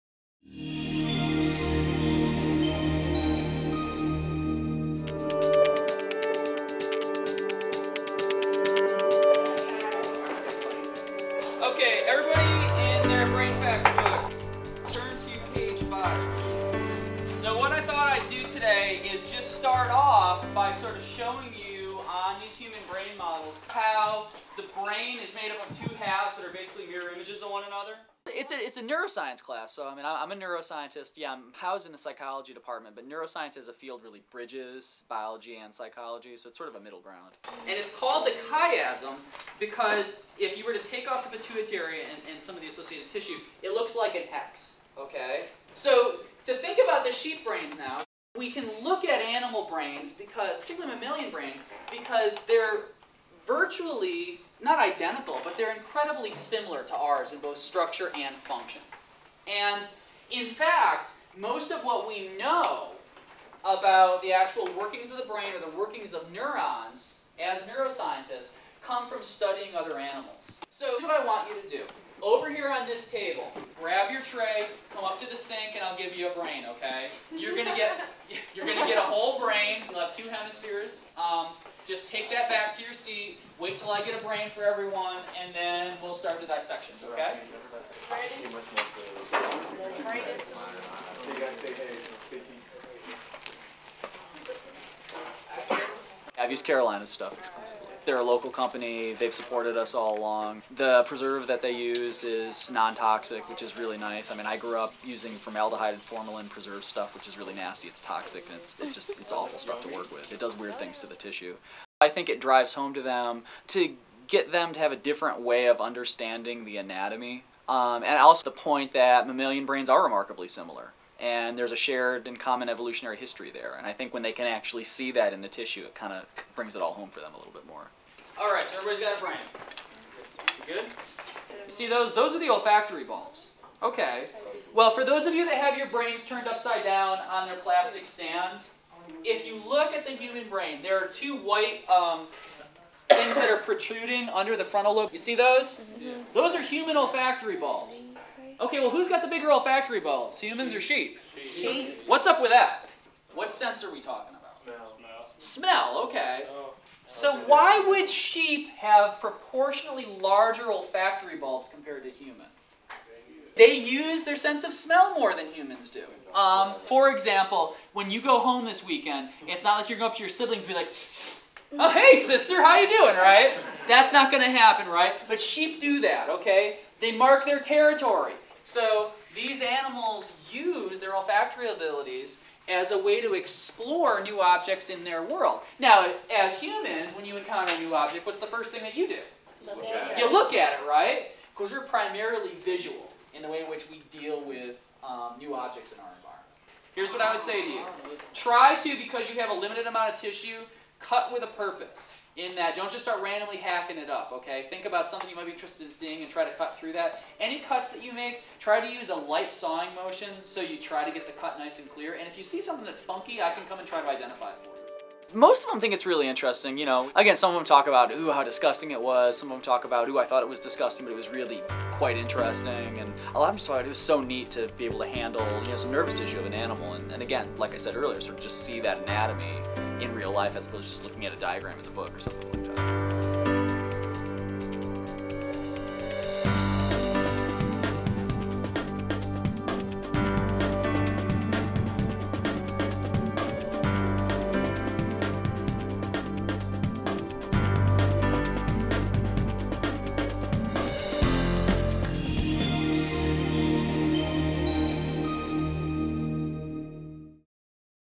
Elon Academy: High School Biology Lab
Carolina Biological Supply donated lab dissection specimens and supplies to the 2009 Elon Academy Summer Program hosted by Elon University in North Carolina. This is the third year Carolina has donated Carolina's Perfect Solution® sheep brain specimens and classroom dissection kits to the Academy

This video shows Dr. Mat Gendle, Assistant Professor of Psychology at Elon University, using the specimens in his neuroscience lab with the Elon Academy students.

Carolina and Elon University School of Communications partnered to create this video.